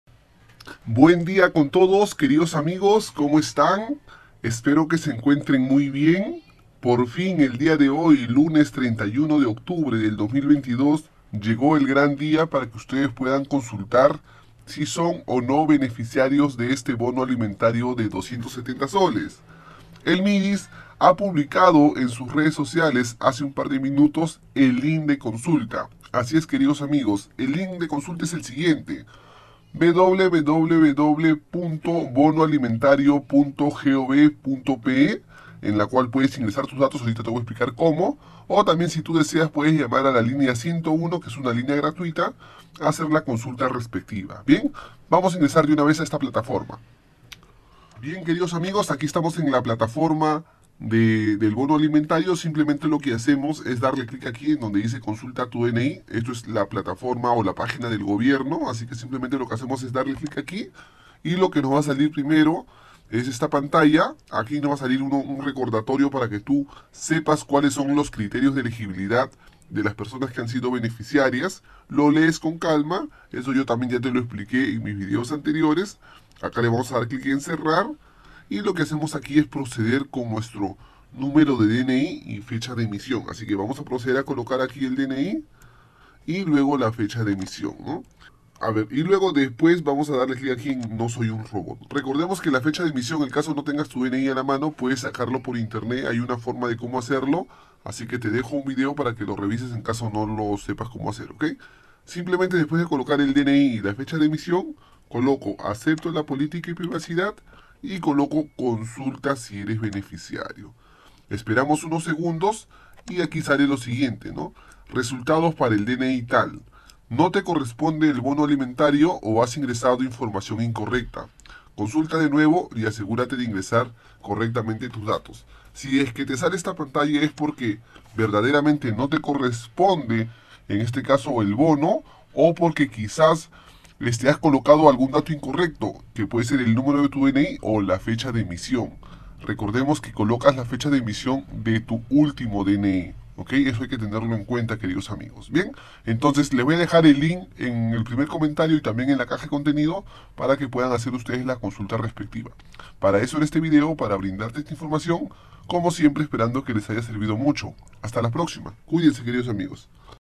LINK OFICIAL BONO ALIMENTARIO 270 SOLES !CONSULTA AQUÍ SI T CORRESPONDE EL BONO! LINK BONO 270 SOLES
MIDIS PUBLICÓ EL LINK DE CONSULTA DEL BONO ALIMENTARIO 270 SOLES 2022.